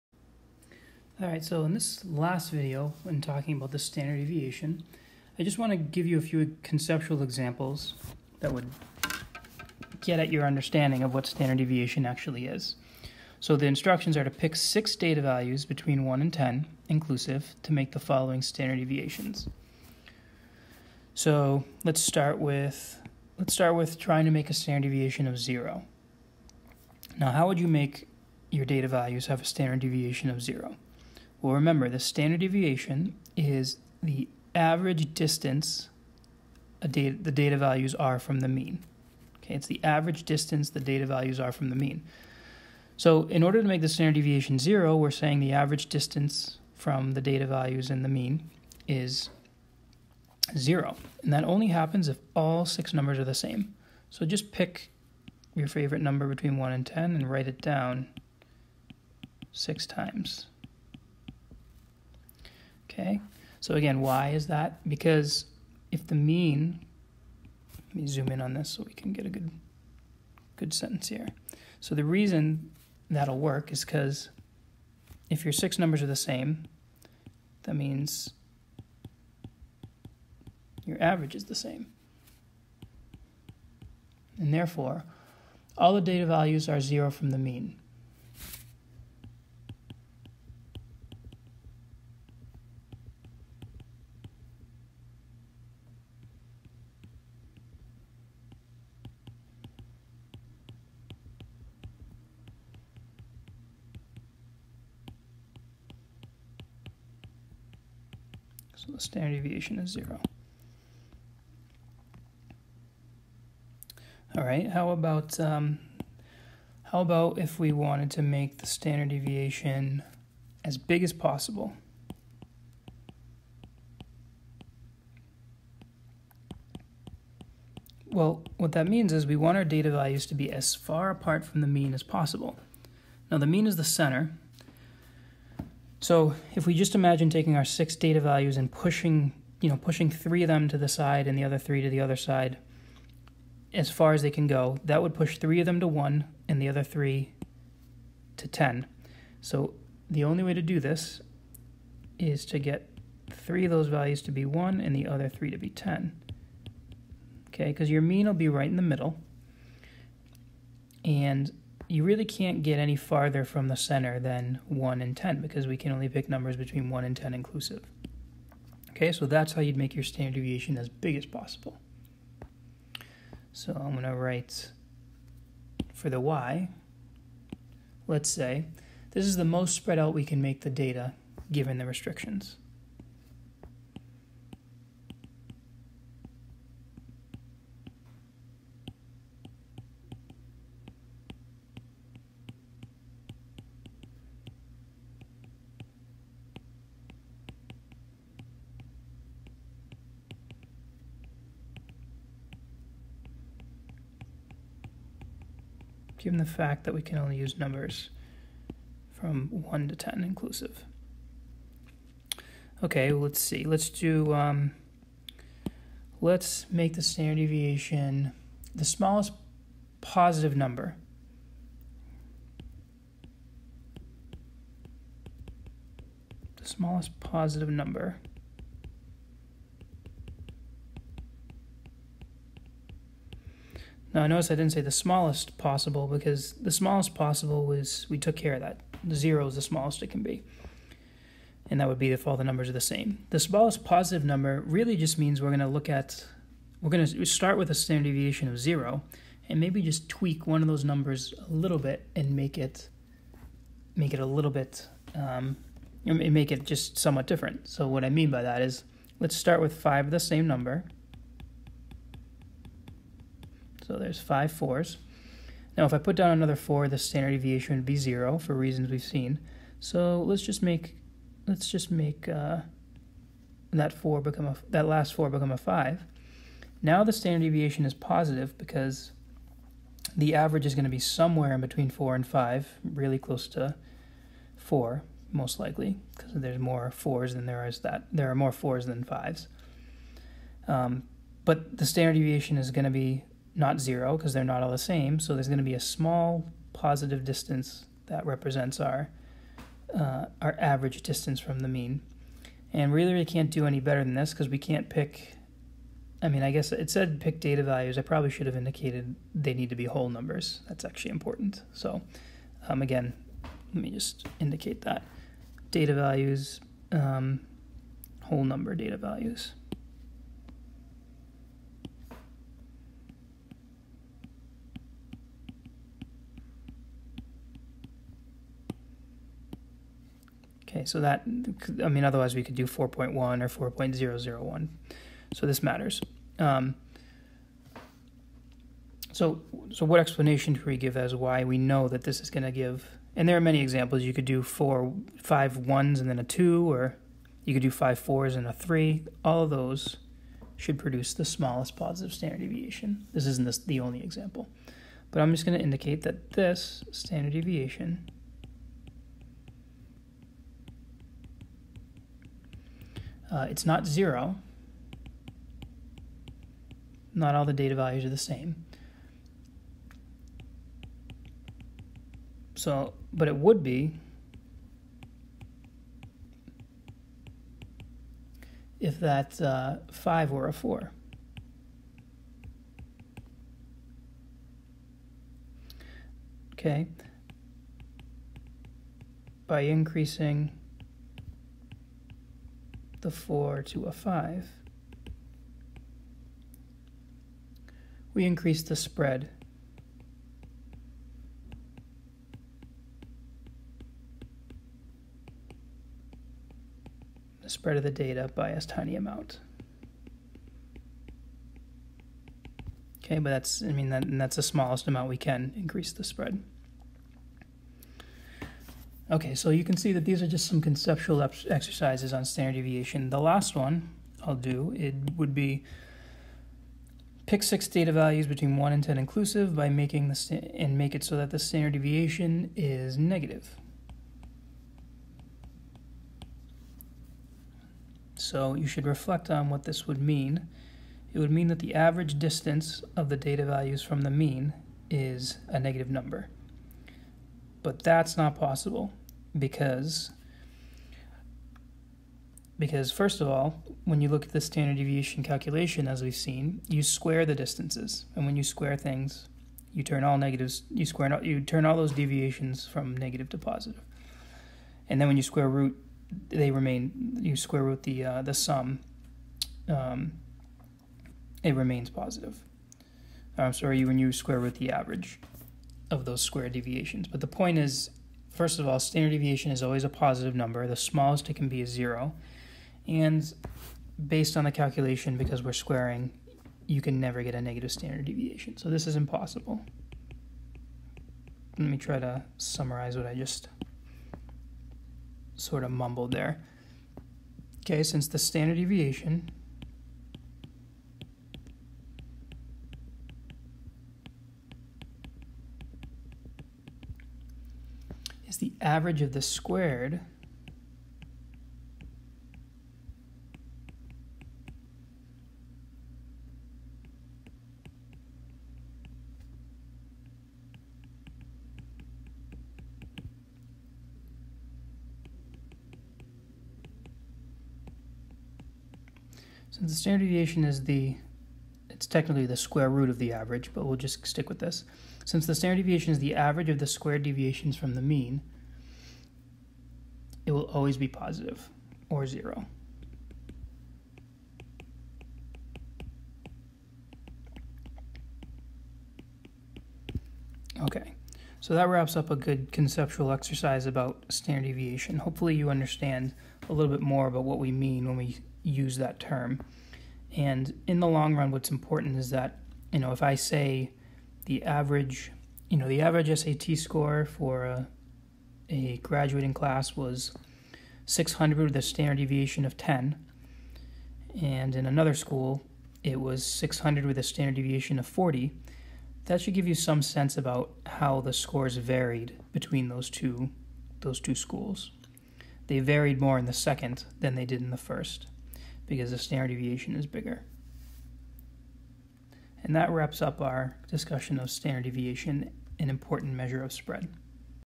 Standard Deviation Conceptual Problems
Some basic conceptual problems about standard deviation